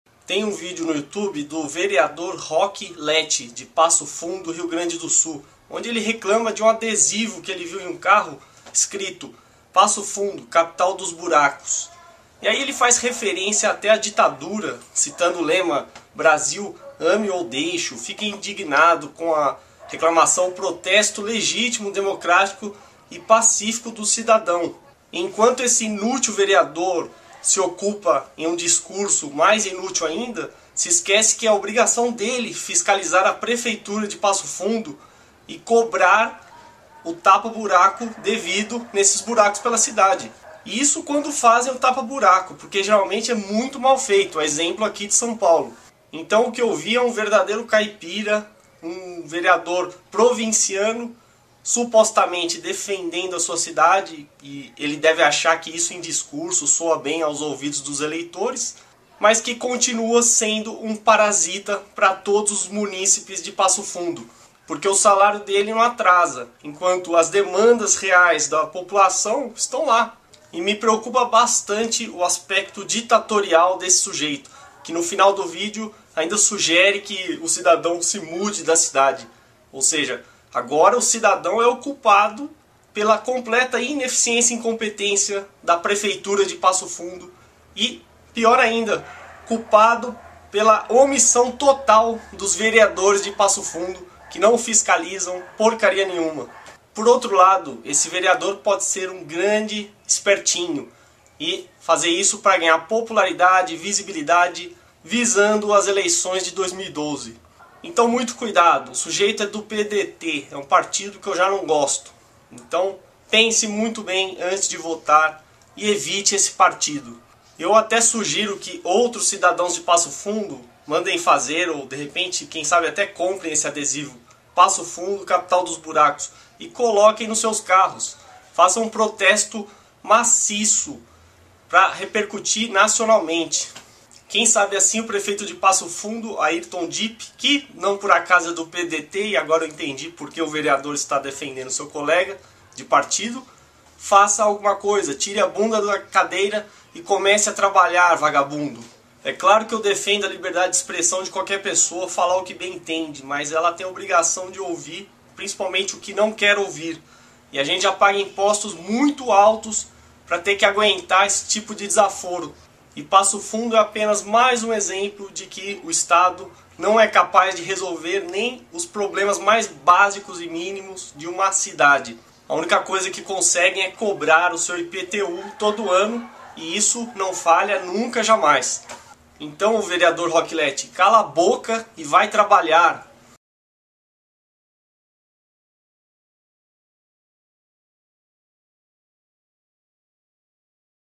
Passo Fundo-RS, a capital dos buracos (e da ditadura)
Vereador Roque Letti ofendendo cidadão passofundense

Declarações de Roque Letti repercutem na internet
Apresentador do CQC compartilhou o vídeo com seus 3 milhões de seguidores. Vereador afirma que pode ter se excedido e defende outras formas de protesto

Excesso de chuva deixa ruas de Passo Fundo esburacadas
Cerca de 60% da cobertura asfáltica da cidade precisa ser refeita

Vejam os projetos de lei importantíssimos de Roque Letti (PDT-RS):

LEI Nº 4564 DE 20 DE JANEIRO DE 2009

DENOMINA DE "SONHO REAL", A RUA INOMINADA QUE INICIA NA RUA 28 DE MAIO E TERMINA NA RODOVIA BR-285, NO BAIRRO JOSÉ ALEXANDRE ZACCHIA

(Do Vereador Roque Letti)

LEI Nº 4535 DE 07 DE NOVEMBRO DE 2008

DENOMINA DE ANTONIO FERNANDES MACHADO - TIO TONHO - O CENTRO DE ATENDIMENTO INTEGRADO A SAUDE - CAIS DO BAIRRO JOSE ALEXANDRE ZACHIA